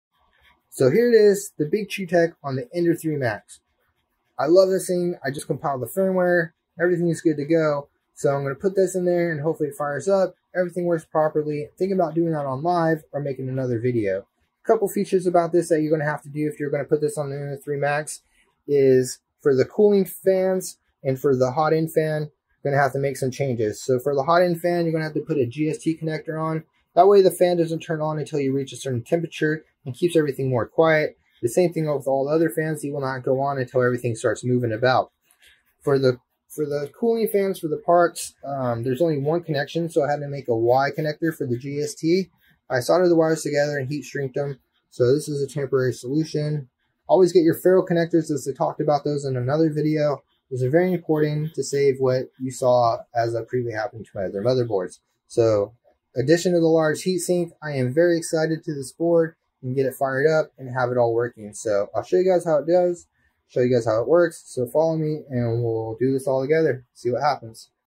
Bigtreetech skr mini e3 v3.0 on Ender 3 max what you need for install and my Firmware
Ender 3 max with the bigtreetech skr mini e3 v3.0

Bigtreetech Mobo‐

JST connections
Need to make Y connection for cooling fans
Jst connector on heat block fan

Ferrule crimping Kit

Solder seal wire connectors are easy. I like to solder and heat shrink myself but if you don't here are those connectors

Firmware
Change estep to 93
adjust BL touch if on right side turn to +48
I have a microswiss DD setup
suggest moving bl touch to left side

Help me out please consider I don't have sponsorship $1 $5 $10 $20 $100 lol

Thank you all for the support please leave feedback Like and follow.